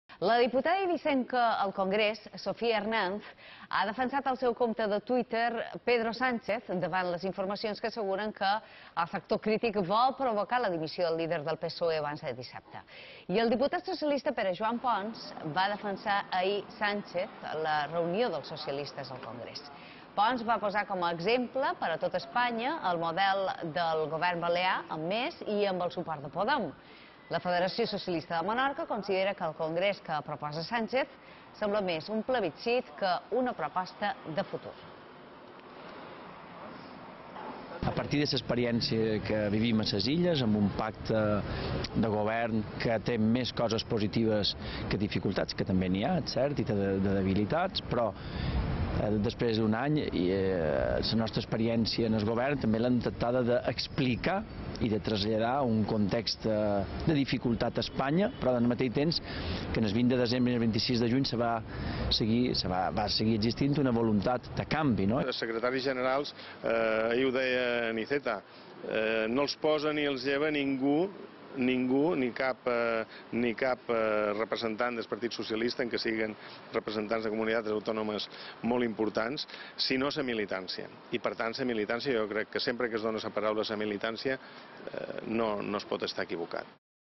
El PSOE de Menorca demana que se treballi per aconseguir el pacte d’esquerres
Des de Menorca, el secretari general del PSOE insular demana que se segueixi treballant per aconseguir el pacte d'esquerres i evitar les terceres eleccions. Pel que fa a la crisi del PSOE, Vicenç Tur creu que el partit necessita una reflexió serena i no un congrés convocat amb presses i amb aires plebiscitaris.